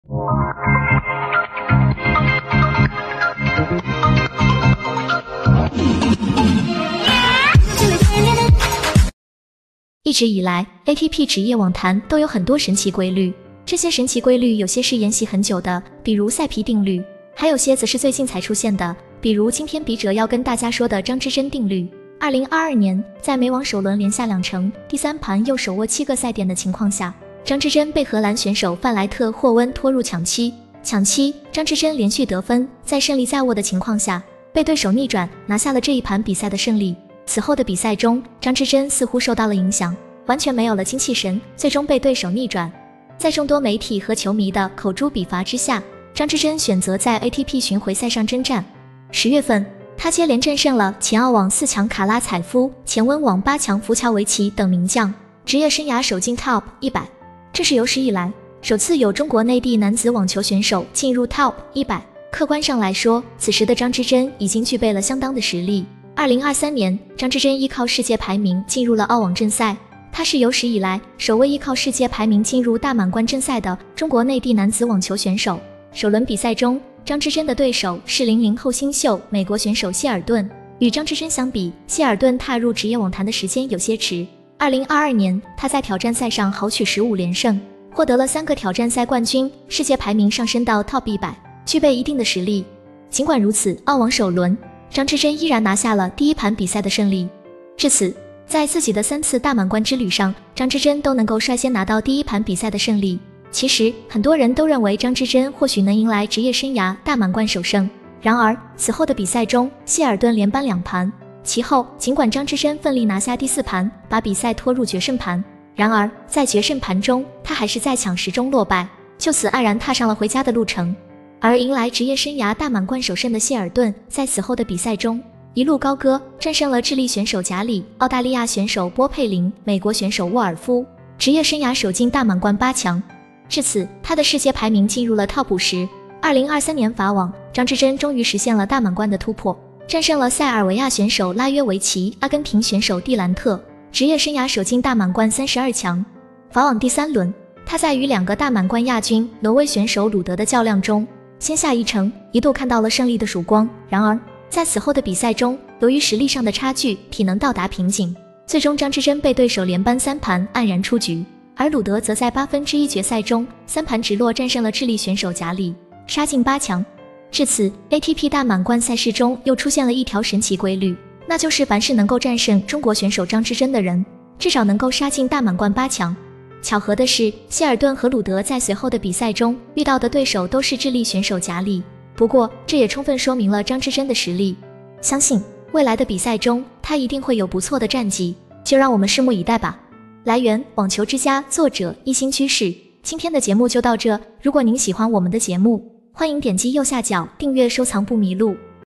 lATP神奇法則再續，2023年誰戰勝張之臻，至少能進大滿貫八強
一直以來，ATP職業網壇都有很多神奇規律。這些神奇規律有些是沿襲很久的，比如塞皮定律；還有些則是最近才出現的，比如今天筆者要跟大家說的張之臻定律。2022年，在美網首輪連下兩城、第三盤又手握七個賽點的情況下，張之臻被荷蘭選手范萊特霍溫拖入搶七。